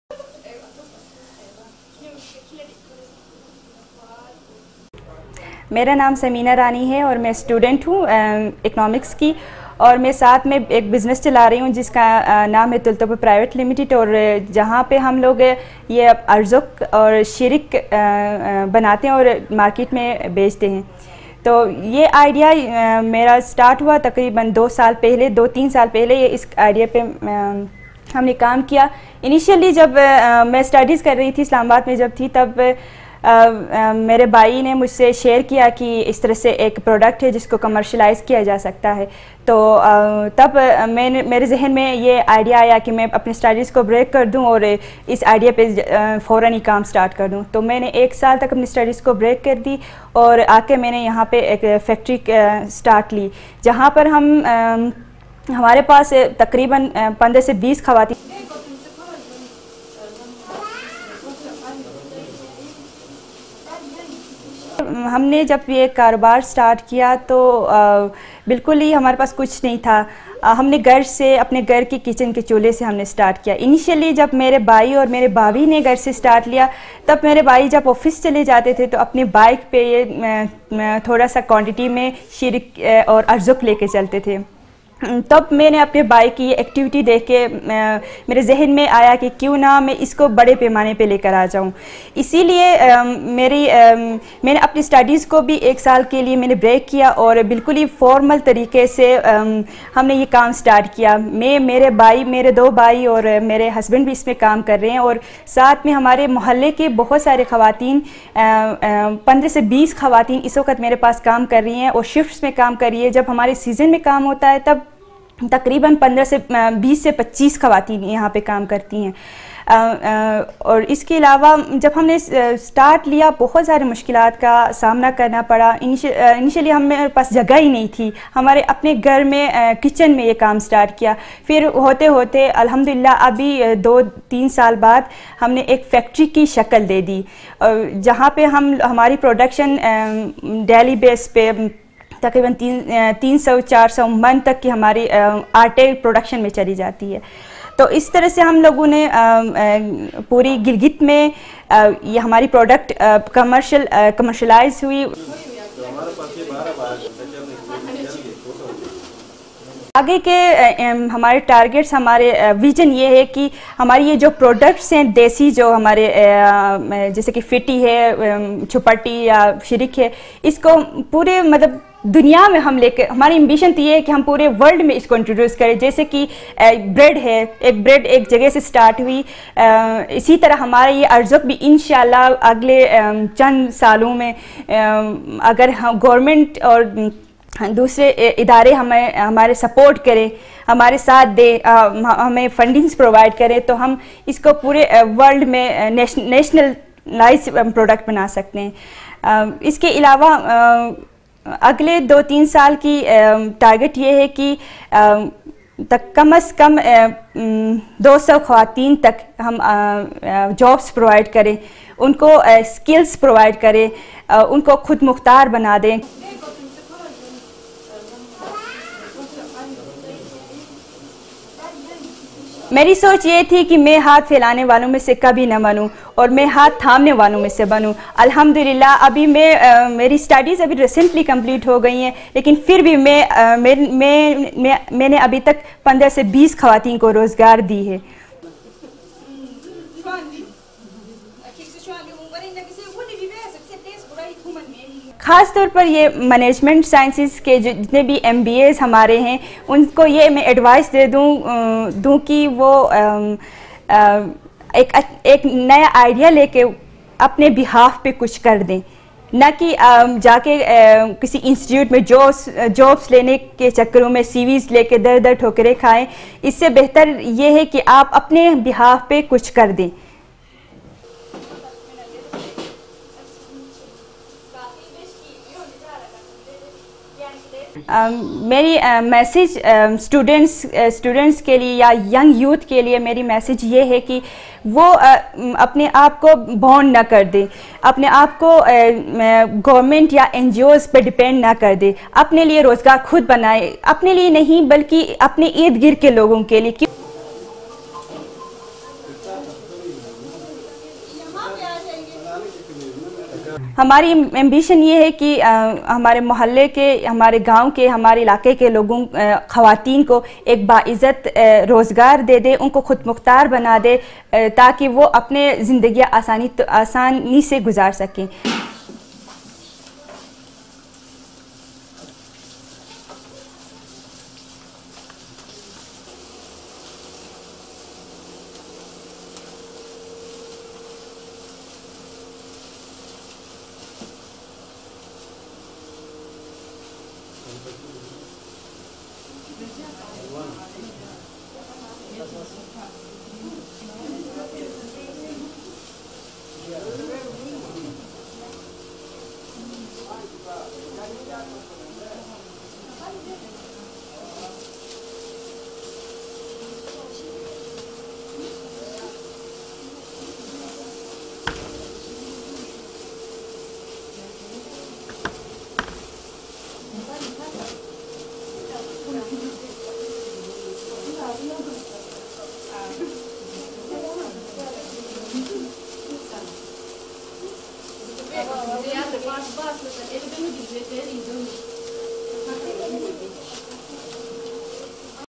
Samina Rani Women Entrepreneurs Share Their Secrets To Success
in this video we will tell you about successfull business women from mountain areas of gilgit baltistan, samnia rani is post graduate from islamabad and now a days doing their own busniess  and also giving jobs to womens of their vicinity and beome successfull busniess women in gilgit baltistan,just watch this mountain girl video and share it to other it will be role model for other womens of mountains areas.